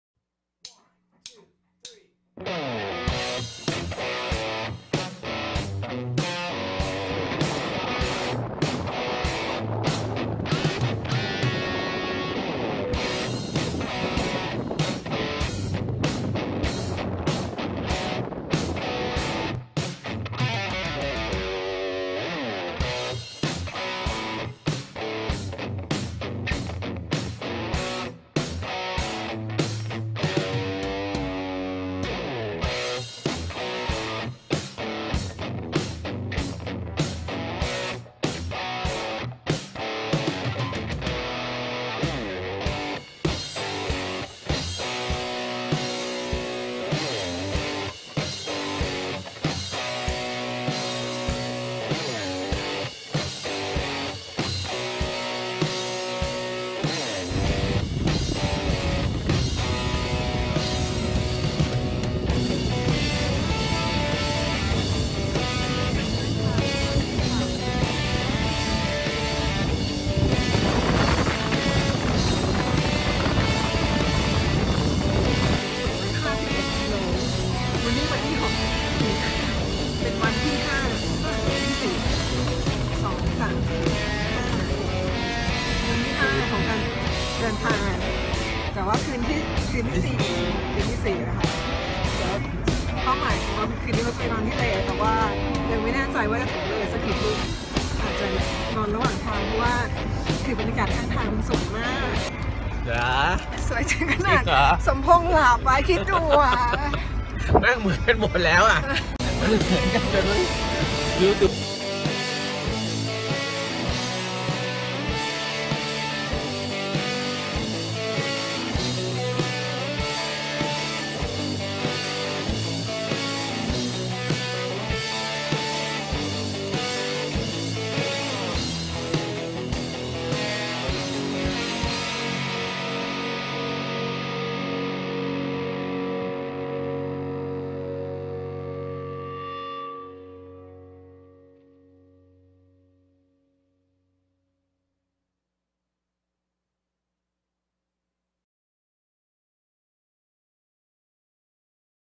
TF Channel: Ladakh - Jammu Kashmir on the Road #2 Manali to Leh
Ladakh - Jammu Kashmir on the Road #2 Manali to Leh

Day1: Delhi
Day2: Delhi to Manali
Day3-5: Manali to Leh
Day 6: Leh-Pangong
Day 7: pangong to Leh
Day 8: Khadunglar pass, Nubra
Day 9: Nubra - Leh
Day 10 : Ladakh
Day 11: Ladakh
Day 12: Ladakh
Day 13: Kargil
Day 14: Rangdum
Day 15 Padum
Day 16: Rangdum
Day 17:
Day 18: Srinagar
Day 19: Agra
Day 20: Delhi
Journery of 3 friends in Leh-Ladakh, Jammu Kashmir and Srinakar in 22 days

This was 3 days 2 nights on the road from Manali to Leh, one night at jispal and one night at Lato, Incredible sceneric view!